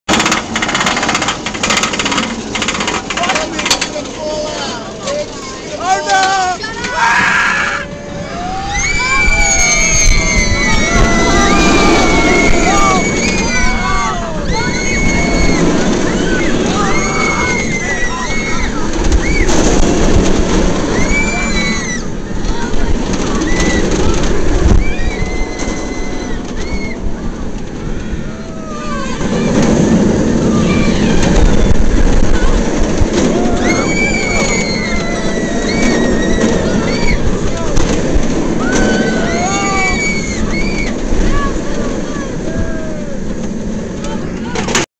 Batman The Ride @ Six Flags Great Adventure
my dad didnt wanna wait for the front ow.